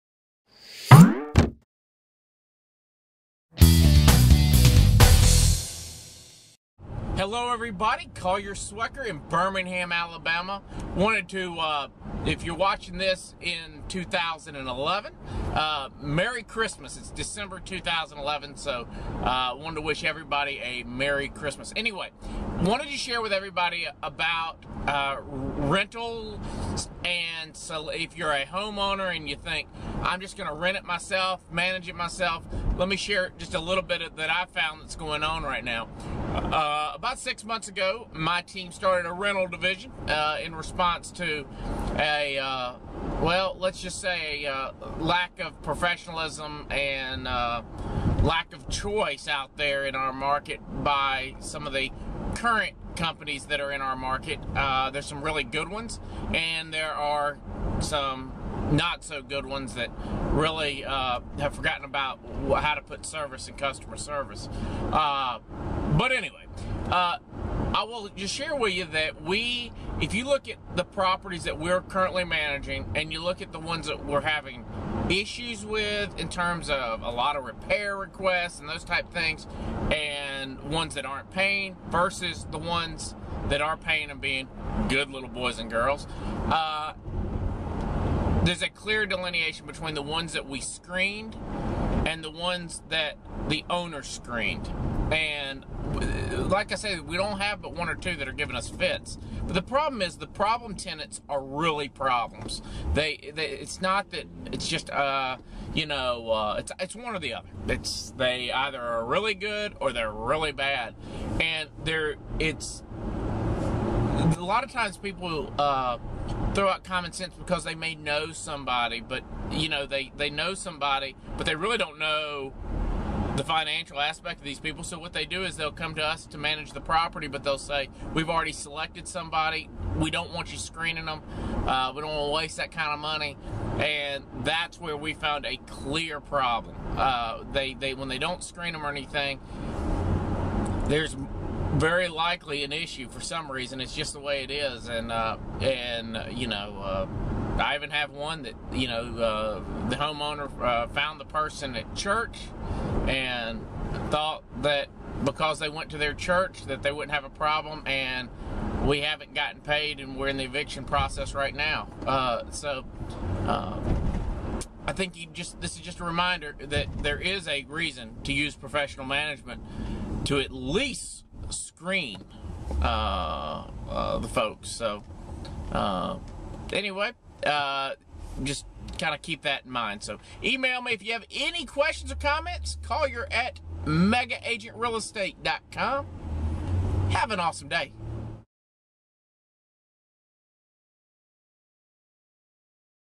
Should You Manage Your Own Rental Home
Collier Swecker video blogs about why most people need to hire a professional manager to handle their rental property. Many home sellers who have failed to procure a buyer for their home, have made the decision to lease their home and they attempt to save money by taking on the responsibility to collect rents, manage repairs, and everything else that goes along with owning & managing rental property. I have seen so many homeowners that take the self managed route, regret the decision because they end up spending countless hours chasing unpaid rent, making repairs, and spending tons of money and time trying to please the tenants. A professional manager is worth every dime that you spend on them and they will allow you peace of mind in knowing that you don't have to answer that midnight call because the toilet won't stop running.